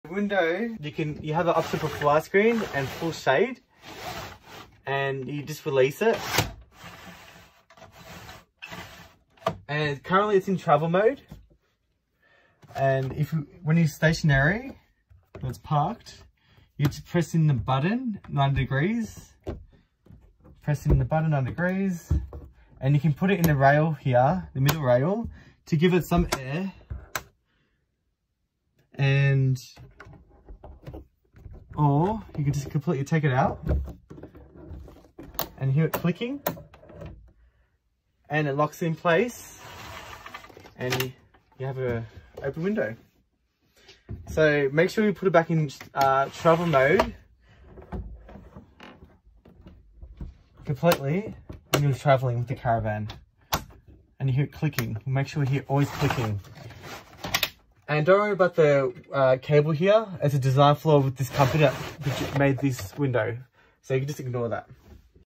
Operate Windows including flyscreen and shade - Malibu Escape 2019 Caravan
How to operate the windows inside for travel and stationary positions